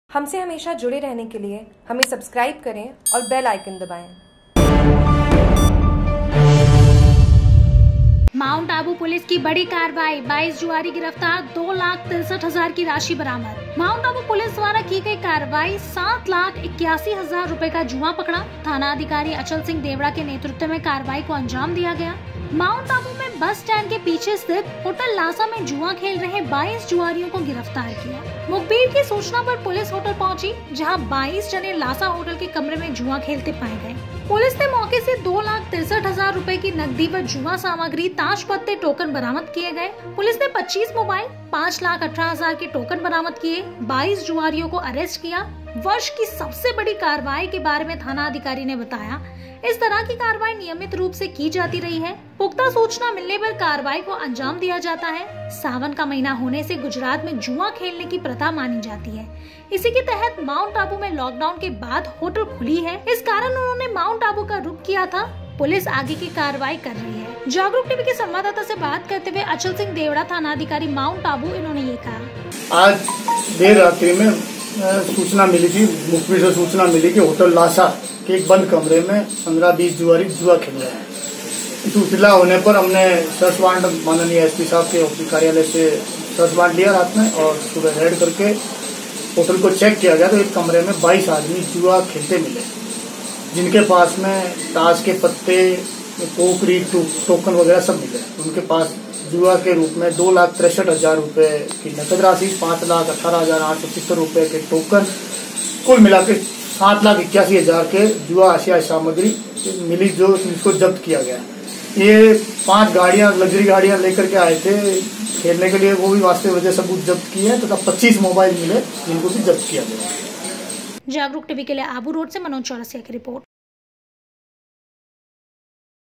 Jagruk TV : माउंट आबू पुलिस की बड़ी कार्रवाई, 22 जुआरी गिरफ्तार 2 लाख 63 हजार की राशि बरामद
आबूरोड। माउंट आबू पुलिस द्वारा की गई कार्रवाई 7 लाख 81 हज़ार रुपए का जुआ पकड़ा। थानाधिकारी अचल सिंह देवड़ा के नेतृत्व में कार्रवाई को अंजाम दिया गया। माउंट आबू में बस स्टैंड के पीछे स्थित होटल लासा में जुआ खेल रहे 22 जुआरियों को गिरफ्तार किया।

 वीओ: मुखबीर की सूचना पर  पुलिस  होटल पहुंची। जहां  22 जने लासा होटल के कमरे में जुआ खेलते पाए गए। पुलिस ने मौके से ₹
2 लाख 63 हज़ार रुपए की नकदी व जुआ सामग्री, ताश पत्ते, टोकन बरामद किए गए।
विओ1: पुलिस ने  25 मोबाइल 5 लाख 18 हज़ार के टोकन बरामद किए।  22 जुआरियो को अरेस्ट किया। वर्ष की सबसे बड़ी कार्रवाई के बारे में थानाधिकारी ने बताया इस तरह की कार्रवाई नियमित रूप से की जाती रही है। पुख्ता सूचना मिलने पर कारवाई को अंजाम दिया जाता है।  सावन का महीना होने से गुजरात में जुआ खेलने की  प्रथा मानी जाती है। इसी के तहत माउंट आबू में लोक डाउन के बाद होटले खुली है।
 इस कारण उन्होंने माउंट आबू का रुख किया था। पुलिस आगे की कार्रवाई कर रही है।

बाइट: अचल सिंह देवड़ा, थानाधिकारी माउंट आबू।

Jagruk TV